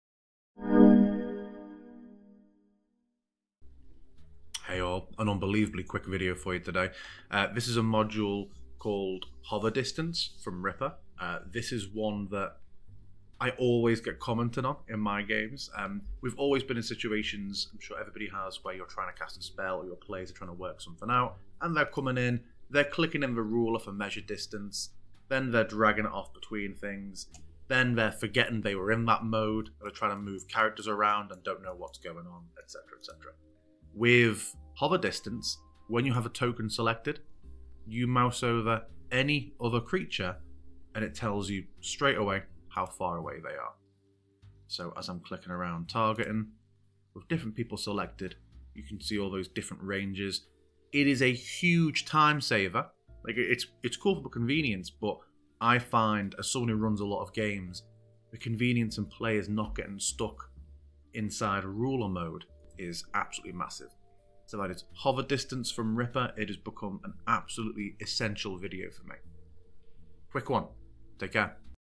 HoverDistance - Immediate distance information in FoundryVTT
A quick video in which I showcase Ripper's "Hover Distance" module, which provides immediately feedback on distance between tokens in FoundryVTT.